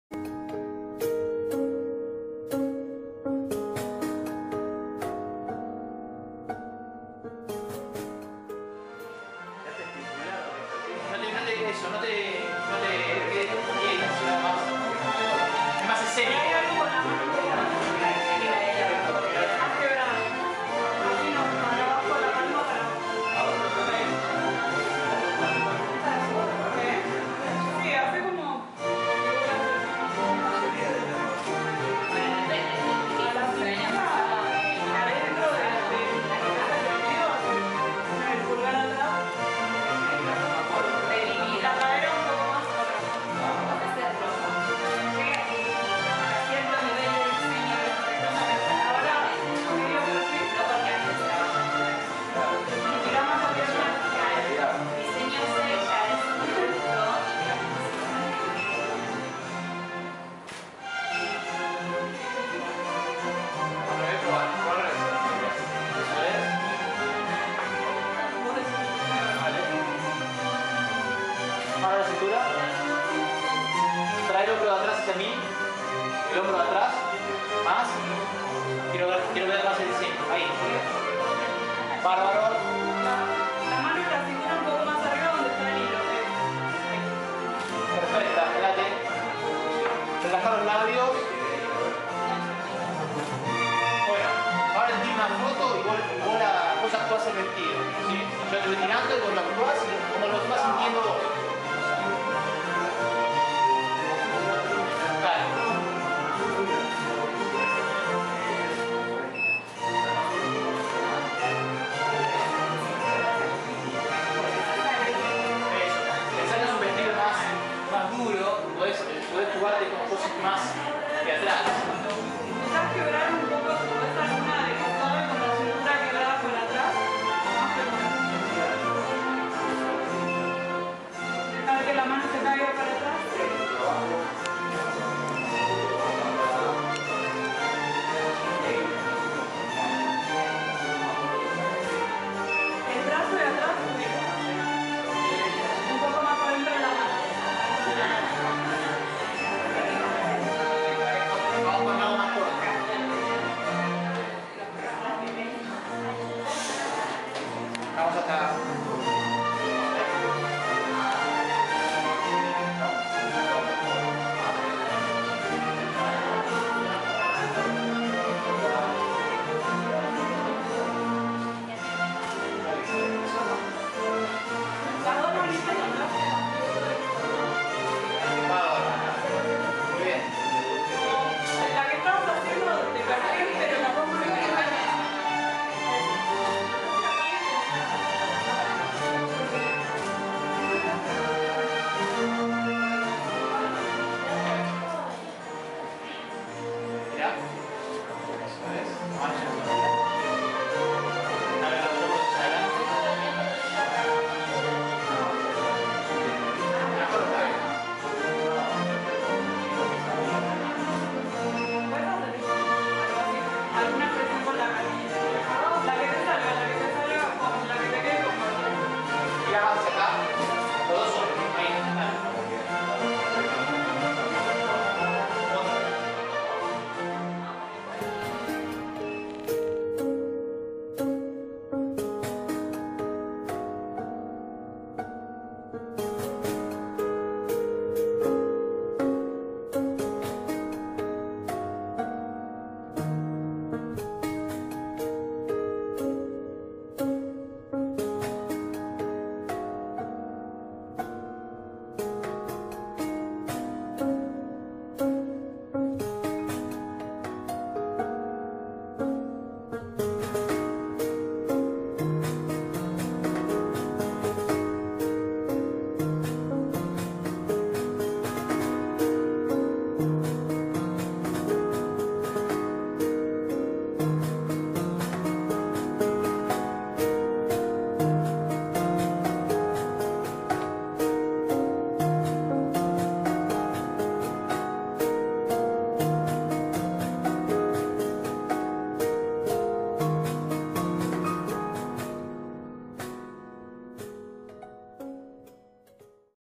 Backstage producción de fotos
Les presentamos el backstage de la producción de fotos de la alumna de la carrera de diseño de indumentaria y textil Florencia Savulsky,  que realizó para presentar dentro del evento Creando Imagen de Moda III que se realizará en el C.C.Borges a partir del 21 de marzo.